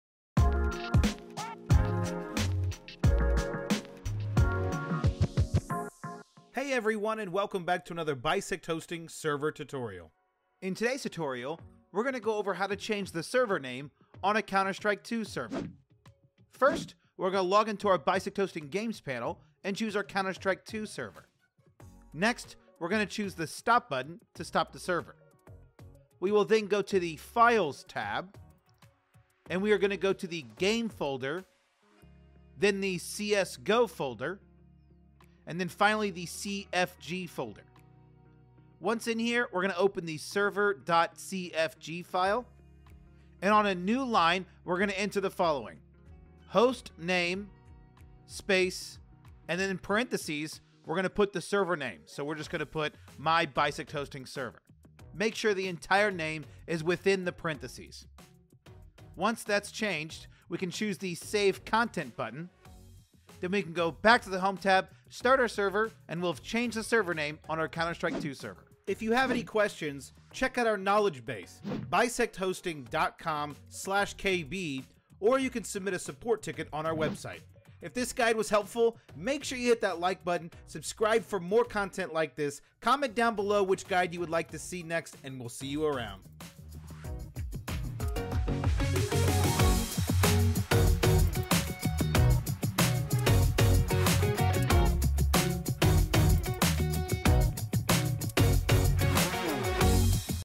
How to Change the Server Name on a Counter-Strike 2 Server!
Learn How to Change the Server Name on a Counter-Strike 2 Server!

Changing the server name on a Counter-Strike 2 server identifies a server quickly and allows server owners to stand out with their names. It can also provide information to players to understand what the server is about.

TIMESTAMPS:

0:00 Intro
0:16 Stop Server
0:25 Editing server.cfg
1:08 Outro